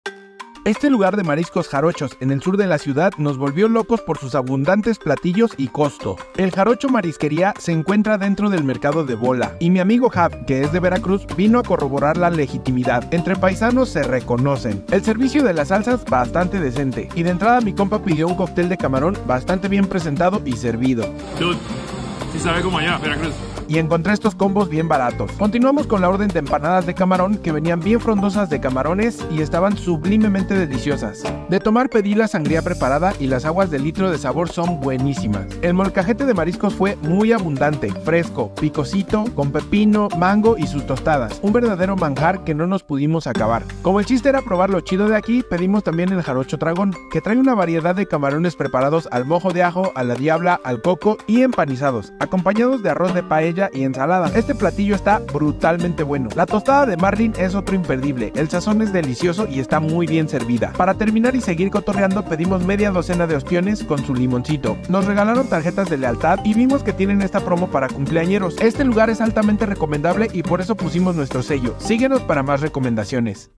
EL JAROCHO MARISQUERÍA - Mariscos jarochos, aprobados por un paladar jarocho, Mercado la bola CDMX
@mar.eljarocho nos encantó por lo bastos que son sus platillos, con un sazón espectacular, y costos muy muy justos y a nuestro parecer, accesibles.

Invitamos a nuestro amigo @javgomez que es de Veracruz, para que probara y nos diera su visto bueno, y pues ya lo vieron, este lugar fue aprobado por un paladar jarocho.